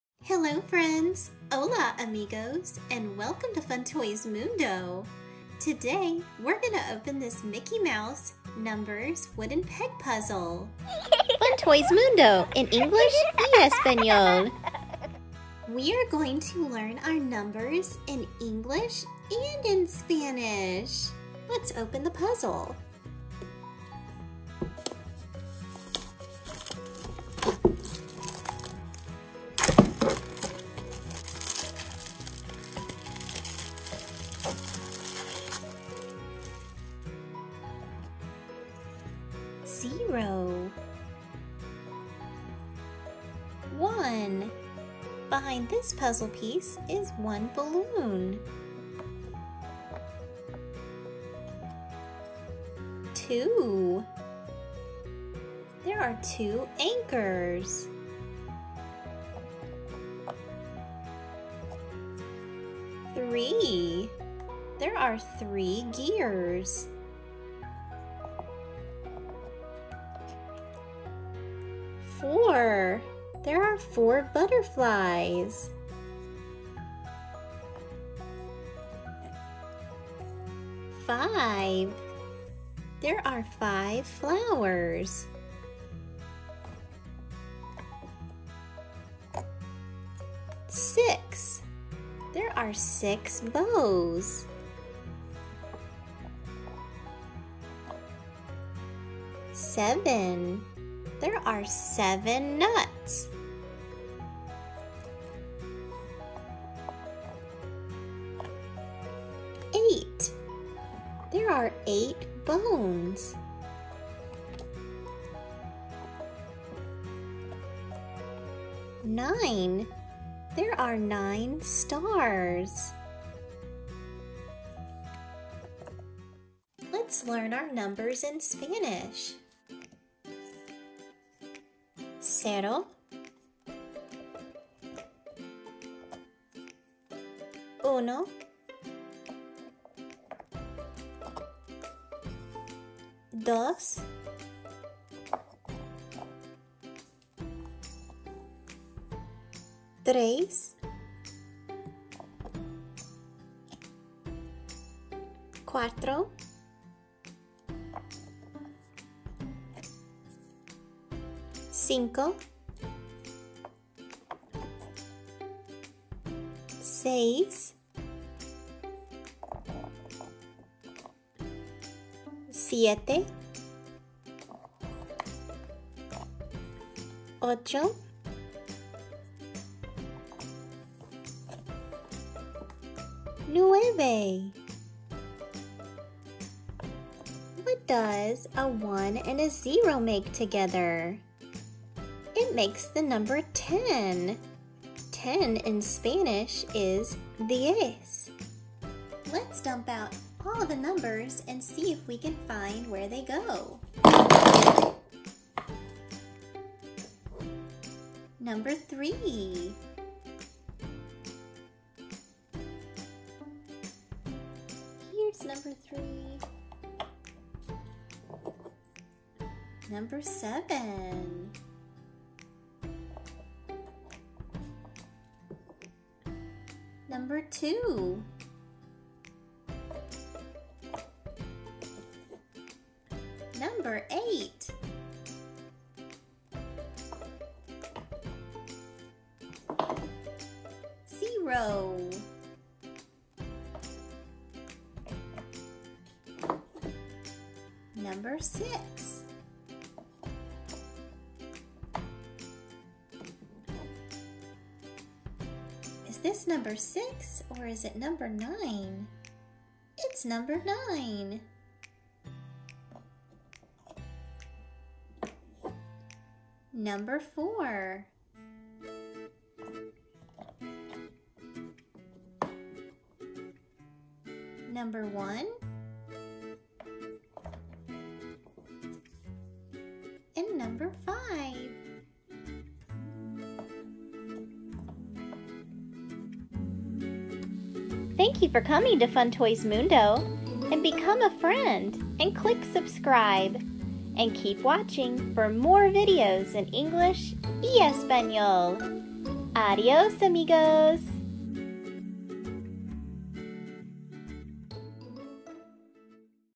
Learn Numbers | English and Spanish | Mickey Mouse Melissa and Doug Numbers Puzzle
Let’s learn numbers in English and Spanish with this Melissa and Doug Mickey Mouse Wooden Puzzle!

🎼🎶Music From...